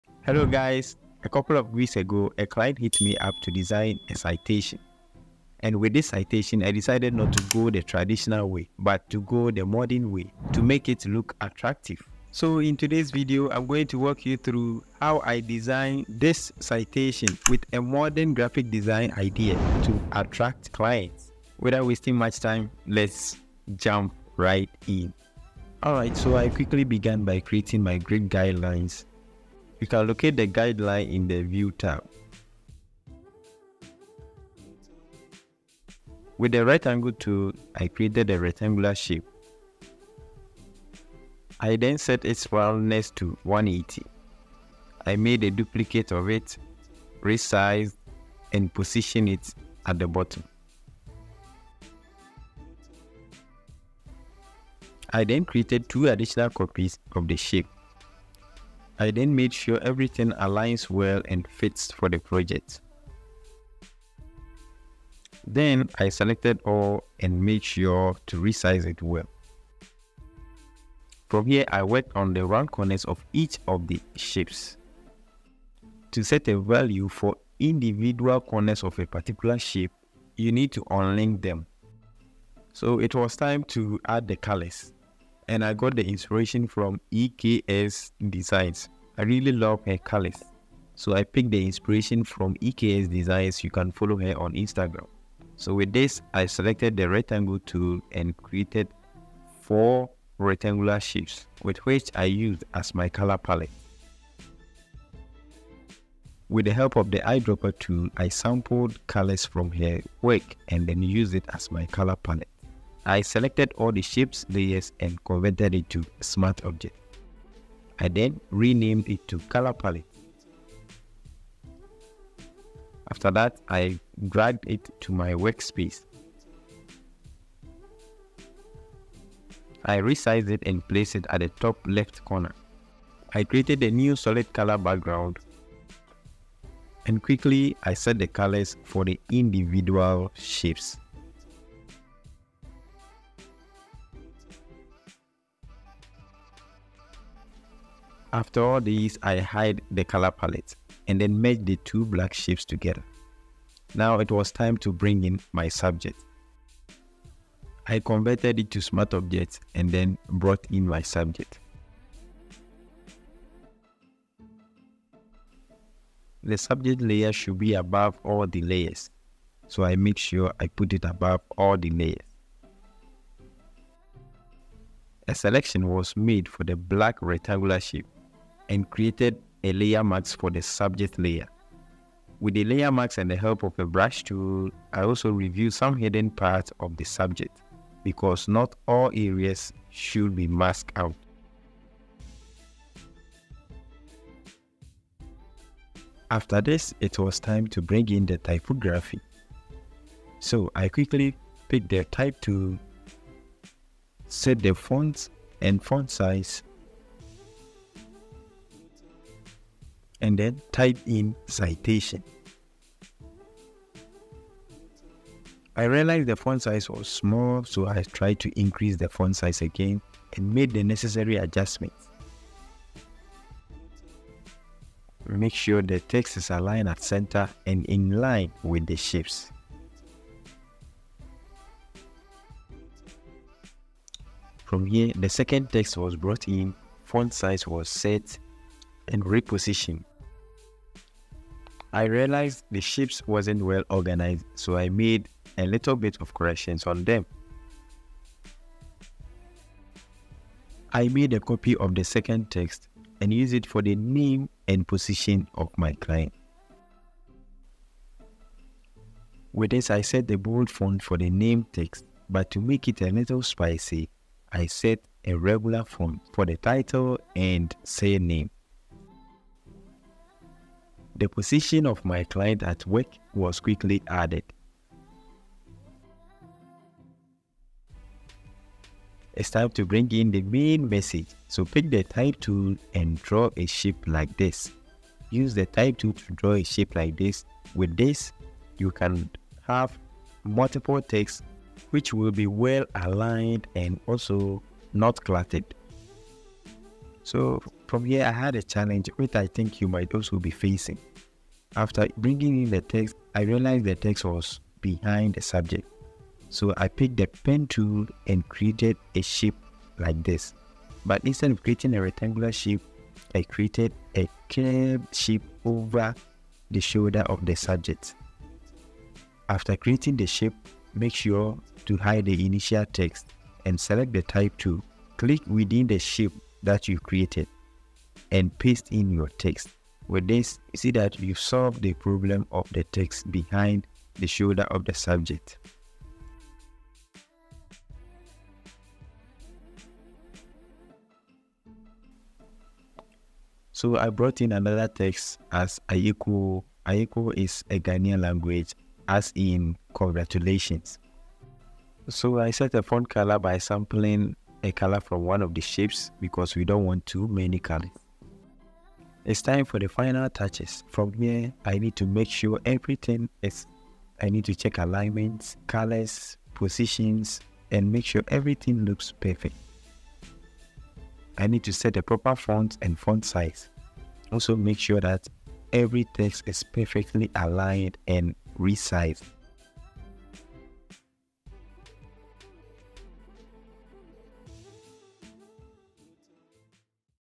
Discover the POWER of Modern Graphics Design in Photoshop!
How to use Photoshop to create stunning graphics designs. This comprehensive guide is appropriate even for those who are new to the process. Additionally, we will go over subjects like citation design, typography, poster design, and graphic design concepts in Photoshop!

►SOFTWARE USED IN THIS VIDEO:
1) Adobe Photoshop cc 2019 (Content)